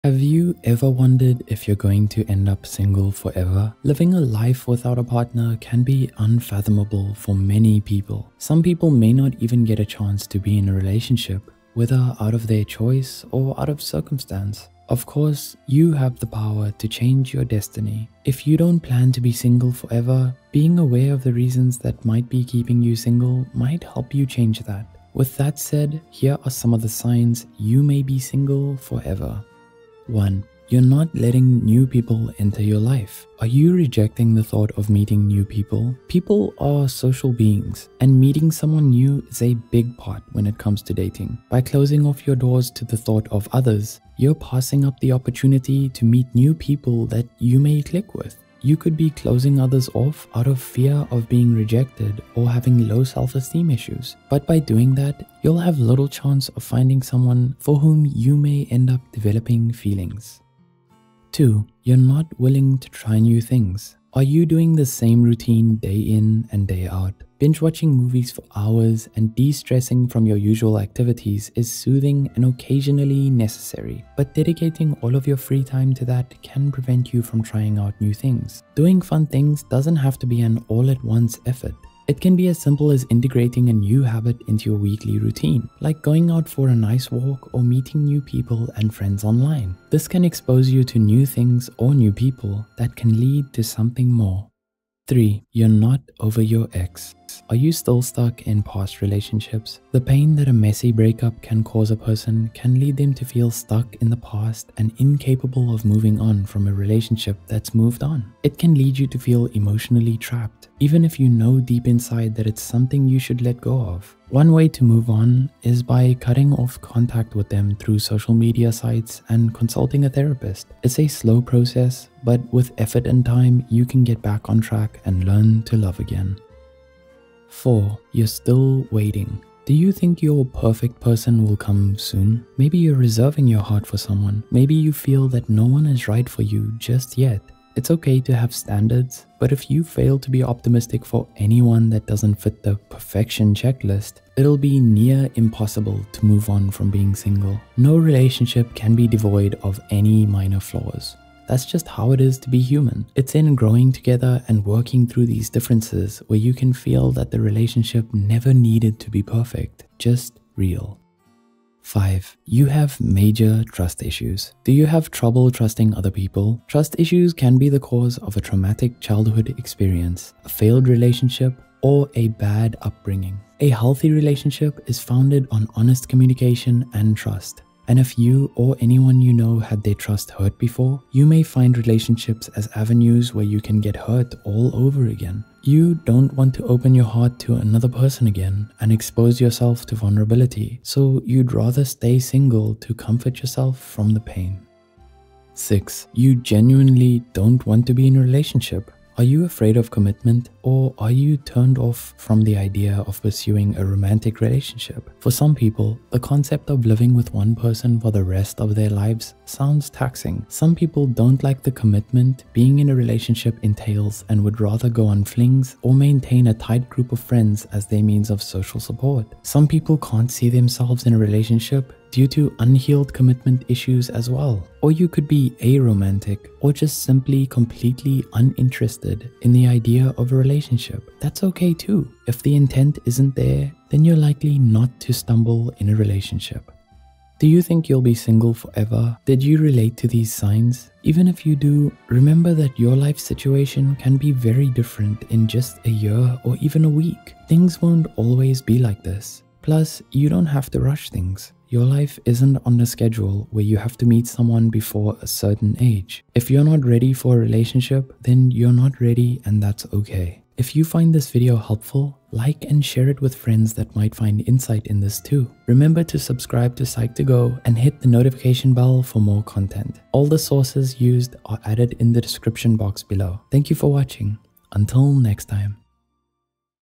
6 Mindsets That Keep You Single For A Long Time
Are you constantly asking yourself, "Why am I single?" It might be time to examine the mindsets that could be holding you back from meaningful connections and keeping you single. Dating can be tough but here are a few common mindsets that might be hindering your chances of finding love and companionship.

Let's build a supportive community focused on personal growth and thriving relationships!

Writer: Joshua Munoz
Editor: Vanessa Tao
Script Manager: Kelly Soong